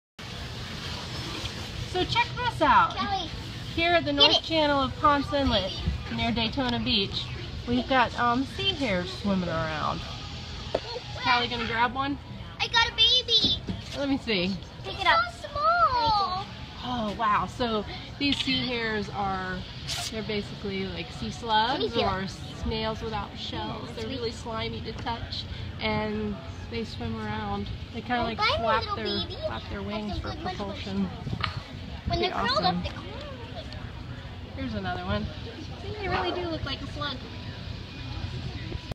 McMermaids Sea hares Ponce Inlet 11Jun2015
Sea hares swimming by the sailboat, Ponce Inlet, Florida. Super slimy. Super awesome.